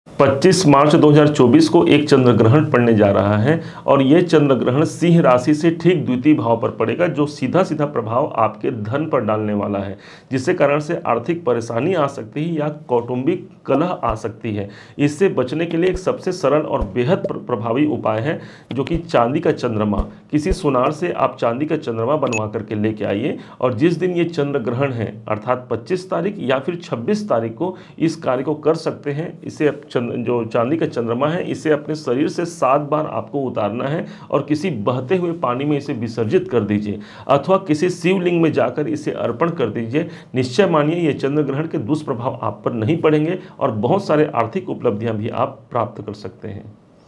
सिंह (Sinh) Leo राशि ये चंद्र ग्रहण बदल सकता है आपका जीवन करें ये उपाय
25 मार्च 2024 को एक ऐसा अद्भुत ग्रह संजोग बन रहा है जिसे चंद्रग्रहण योग कहा जाता है। इस दिन चंद्रमा और केतु एक साथ कन्या पर विराजमान होंगे। हमने तार्किक ढंग से इस दौरान मनोरथ पूर्ण करने वाले एक उपाय इस वीडियो में प्रदर्शित किया है। अतः इस वीडियो को पुरा जरूर देखें, और अपनी कामना सिद्ध करें और अपनी कामनासिद्ध और अपनी और अपनी कामना सिद्ध करें। अतः इस वीडियो को पूरा जरूर देखें कृपया स्किप ना करें।
Dr. Krishna Kant Chaturvedi
Professional Consultation:
Ask your questions in comment if you have